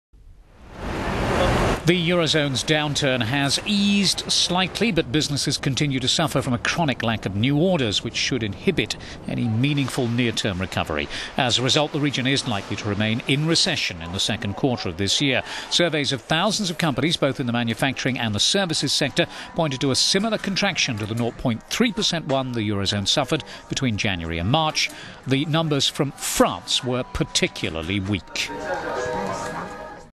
Fewer new orders mean continued eurozone recession - economy
The eurozone's downturn has eased slightly, but businesses continue to suffer from a chronic lack of new orders, which should inhibit any meaningful near-term recovery.

As a result the region is likely to remain in recession in the second quarter of 2013.

Surveys of companies - both in the manufacturing and the services sector - pointed to a similar contraction to the 0.3 percent one the eurozone suffered between January and March.

Markit's flash Eurozone Services PMI, which surveys the purchasing managers at around 2,000 companies ranging from major banks to caterers, rose in May to 47.5, a three-month high, from 47.0 in April.

While that was a little better than economists expected, the PMI has now spent 16 straight months below the 50 mark that divides growth and contraction.

"We see this as confirmation of our expectation that the eurozone economy will end its downtrend in the spring," said Christoph Weil, analyst at Commerzbank.

"That said, a noticeable recovery is still not in sight; the economy will only grow slightly in the coming quarters and it will continue to feel like a recession."

*French business slump*

The numbers from France were particularly weak.

Markit said its preliminary composite purchasing managers index was unchanged in May at 44.3, far below the 50-point line dividing expansions in activity from contractions.

Markit chief economist Chris Williamson said business activity remained far too subdued to declare the end of the downturn in sight.

"There is just a lack of belief that the situation is going to improve any time soon. There is strong dissatisfaction with the government and its handling of the economy."